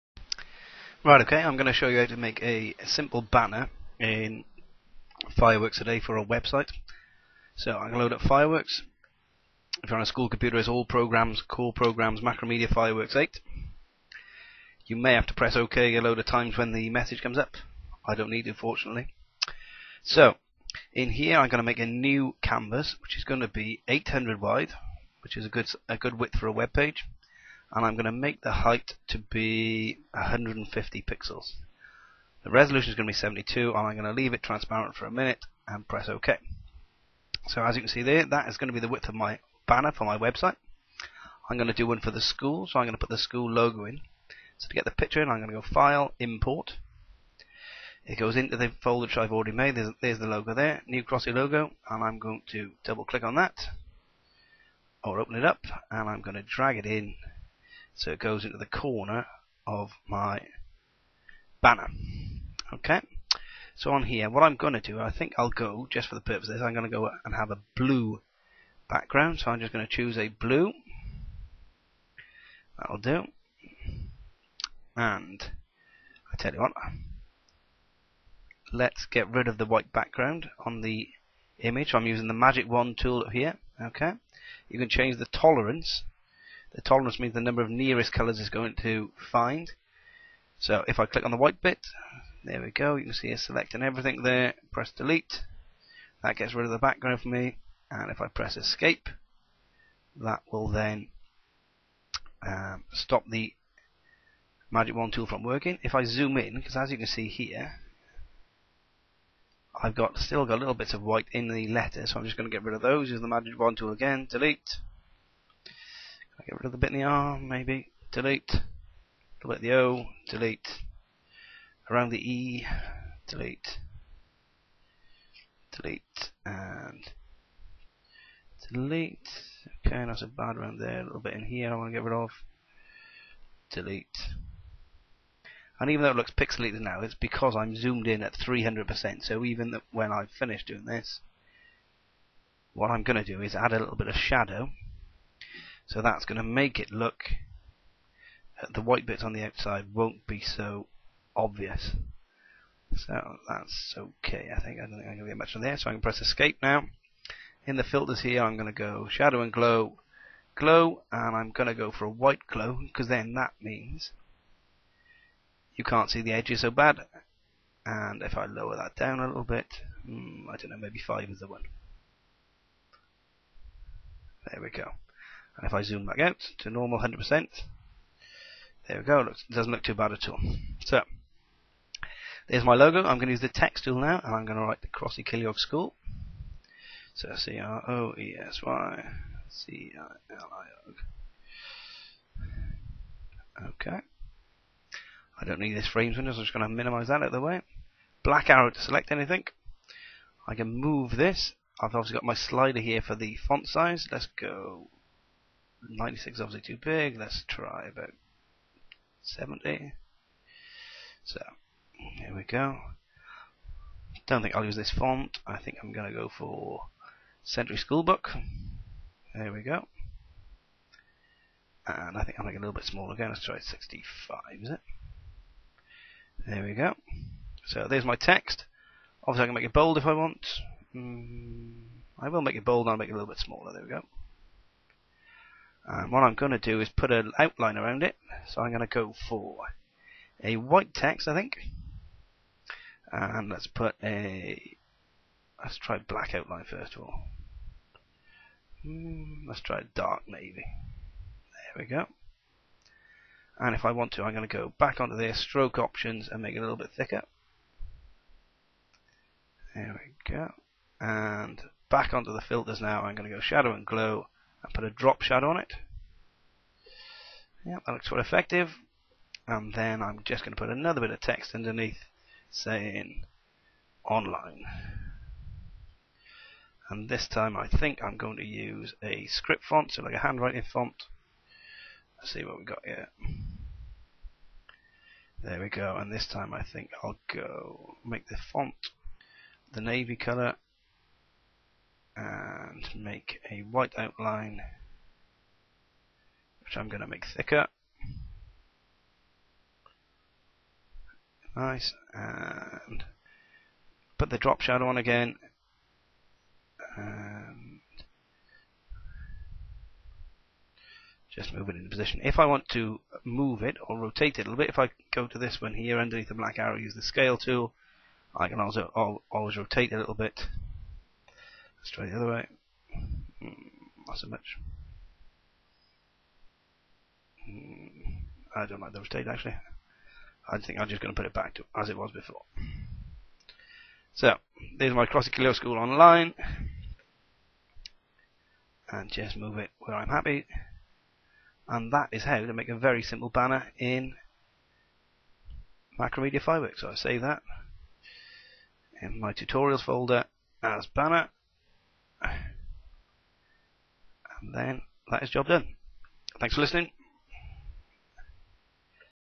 How to create a simple web banner in Macromedia Fireworks 8
This video shows you how to create a simple web banner.  It will show you how to use the magic wand tool to take the background off an image. It also shows you how to add simple effects or filters to images and text.  Fillters include drop shadow and glow.